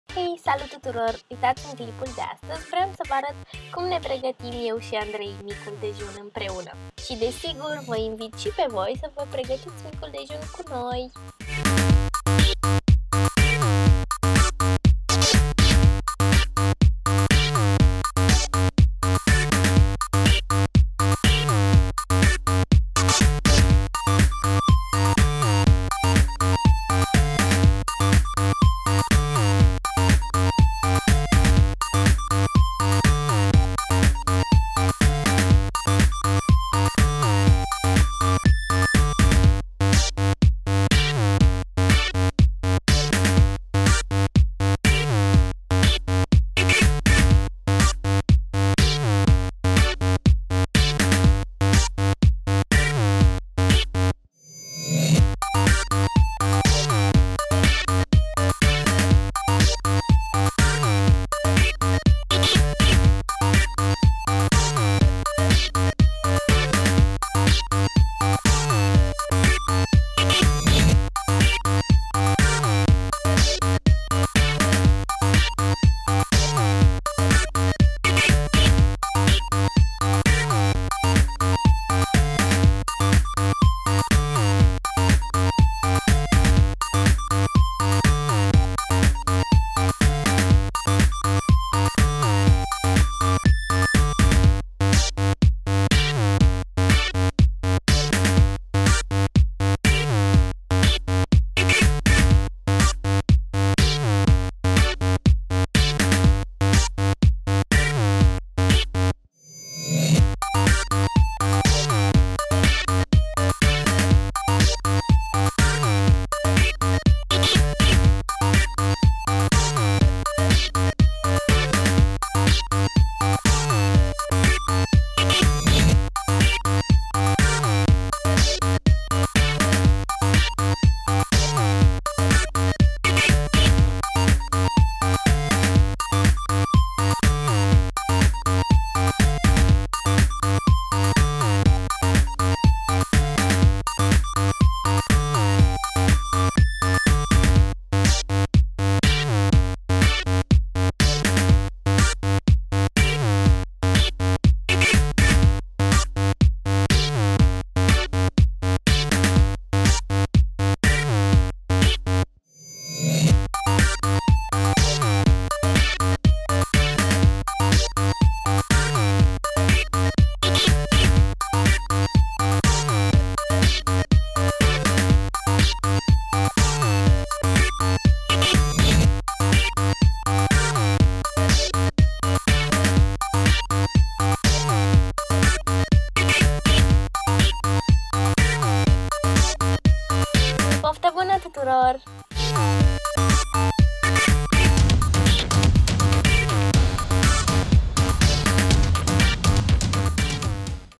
Cum imi prepar micul dejun
OUA+SALAM - ALE LUI ANDREI